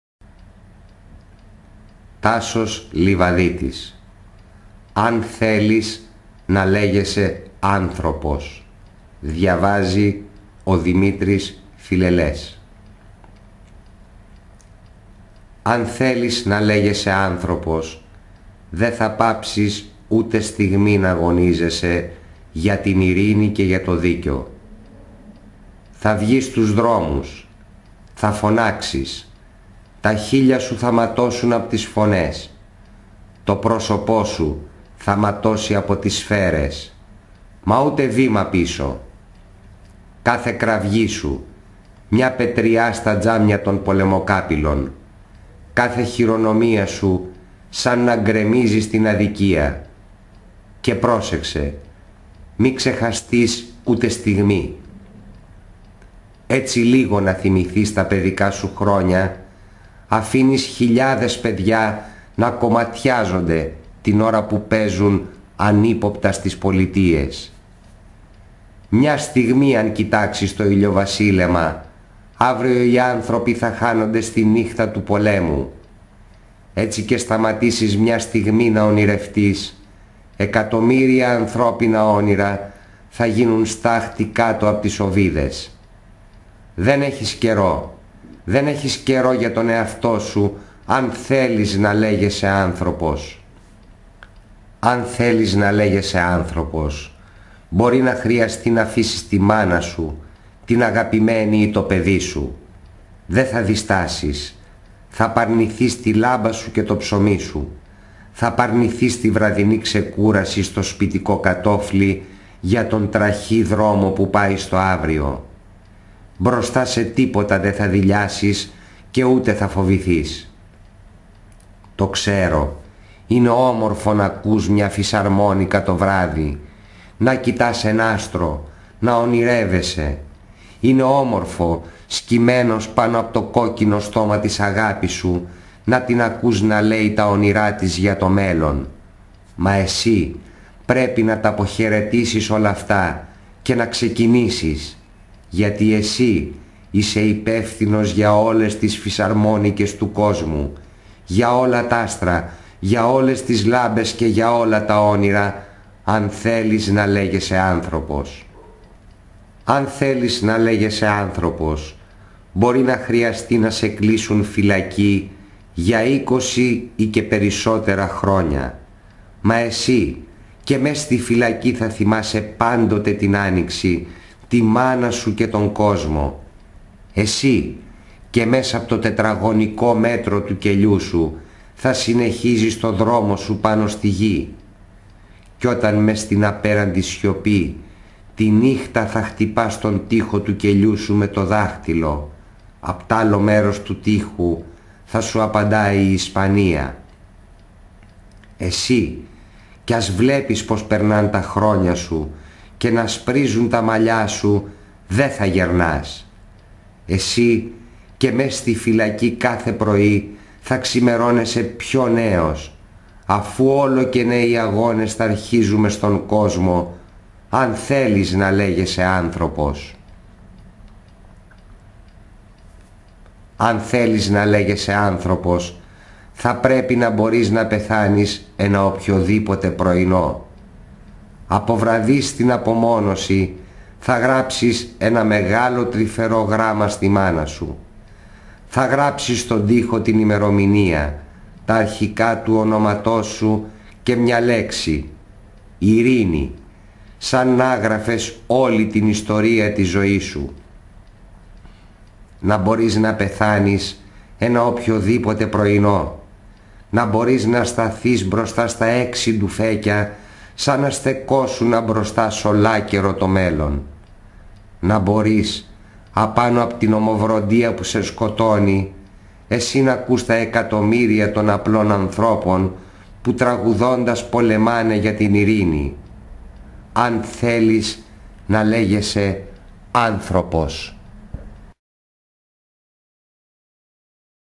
ΤΑΣΟΣ ΛΕΙΒΑΔΙΤΗΣ - ΑΝ ΘΕΛΕΙΣ ΝΑ ΛΕΓΕΣΑΙ ΑΝΘΡΩΠΟΣ
ΑΚΟΥΣΤΕ...
... "Αν θέλεις να λέγεσαι άνθρωπος ", ένα ποίημα του Τάσου Λειβαδίτη που αγγίζει τις πιο ευαίσθητες χορδές της ανθρώπινης ύπαρξης.